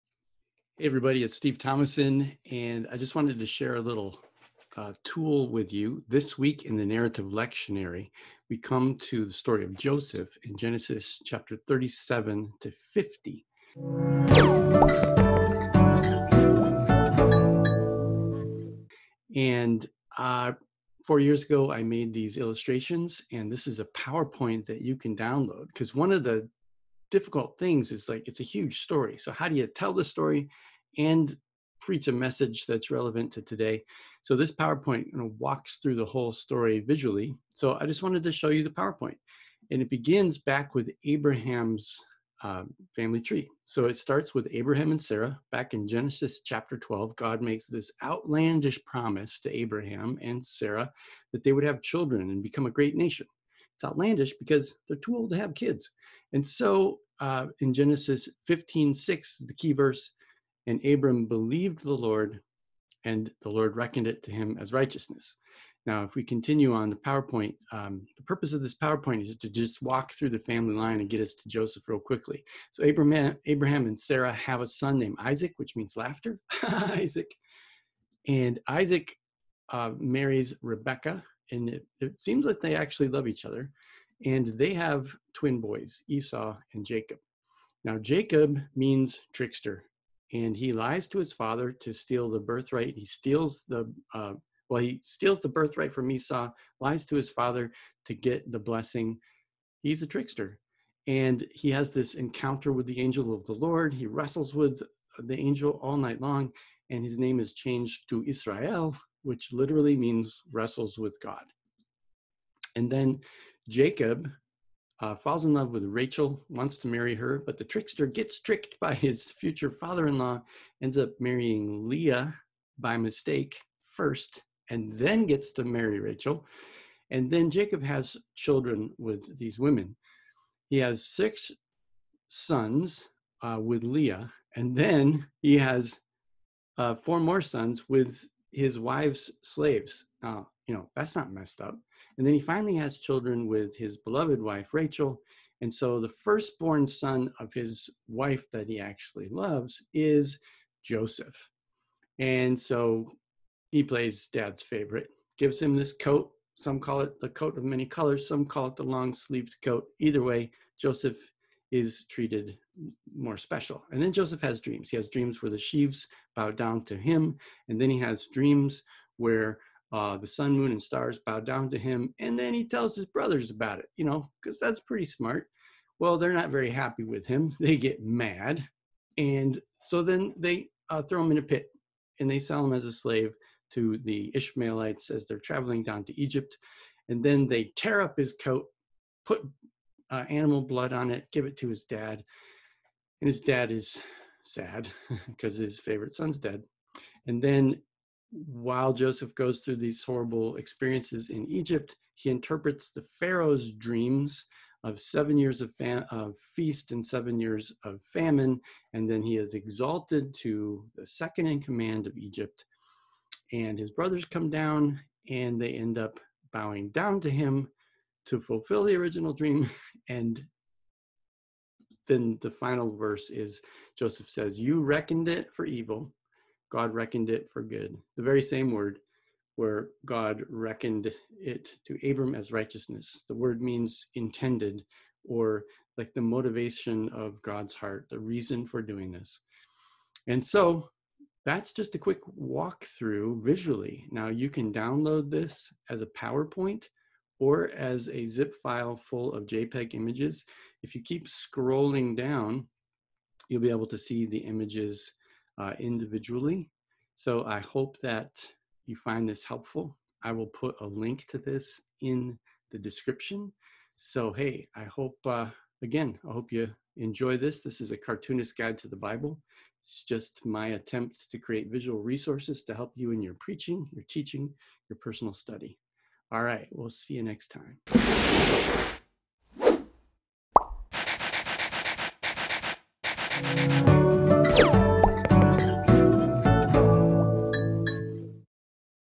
The Story of Joseph in Genesis 37-50
This PowerPoint walks through the story of Joseph in Genesis, beginning with Abraham and Sarah's family tree. This is a messed up family. Yet, in the midst of all the dysfunction, God remains faithful to God's promise to Abraham and Sarah.